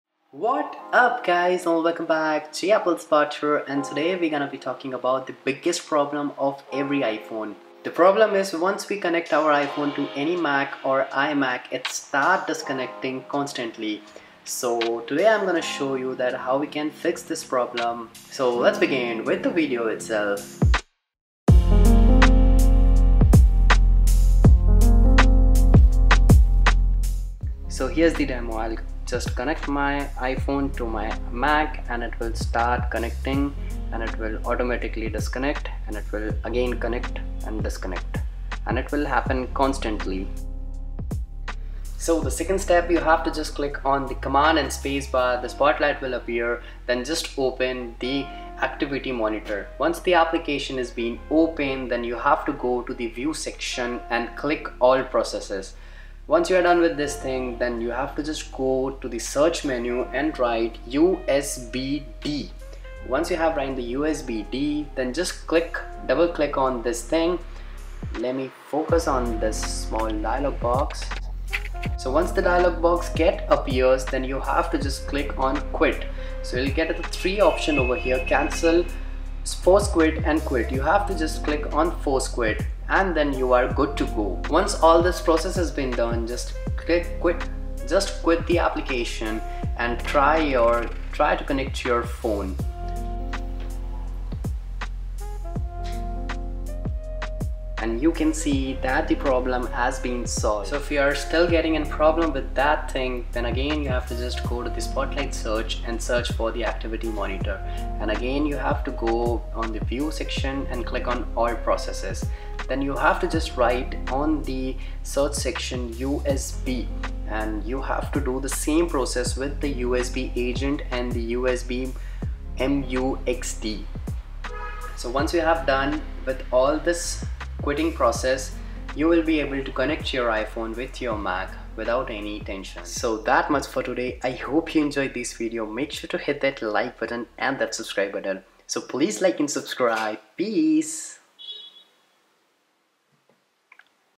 How to Fix iPhone not Connecting to MacBook
How to Fix BIGGEST PROBLEM of iPhone | Why Can't I Connect or Charge my iPhone on my Mac!

This process will help to connect your iphone to MacBook or iMac

Now you. guys can get rid of this biggest problem of iphone.
Steps
1. Connect Your I phone to Mac(This will show you a buzz that you phone is not charging)

2. Click on spotlight search or press space+Command button.

3. Write activity monitor on the search bar and press enter.

4. After the launch of activity monitor on top left hand click  on
view-All Process.

5 On search panel write "USBD" and click on this process after clicking on this click on Force Quit and try and reconnect your I phone it will start charging.